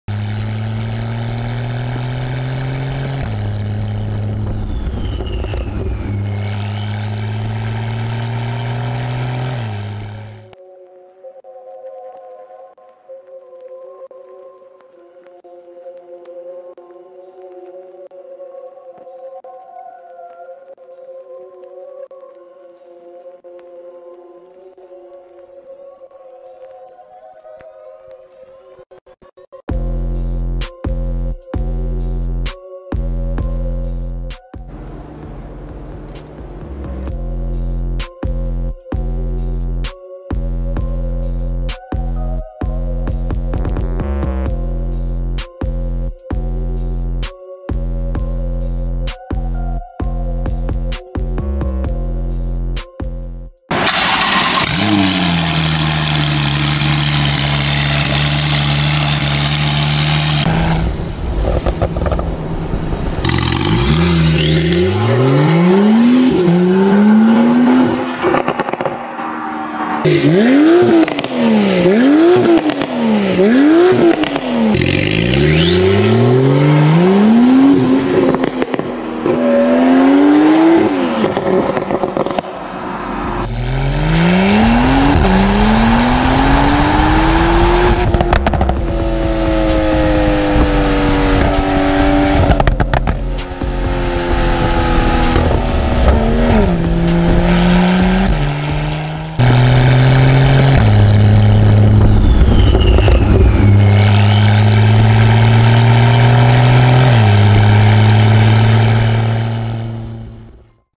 991.2 Carrera S BACKFIRE w/ Valvetronic Fi EXHAUST Sport Bumper Version X One Belief
991.2 Carrera S is installed with Fi EXHAUST Sport Bumper Version valvetronic catback and catless downpipes for an enhanced performance and sound.

Thanks to automotive customization shop One Belief Tyre & Wheels (Malaysia).

00:00-00:54 Intro, Install
00:54-01:01 Cold Start
01:01-01:46 Launch, flyby
01:46-01:51 Fi EXHAUST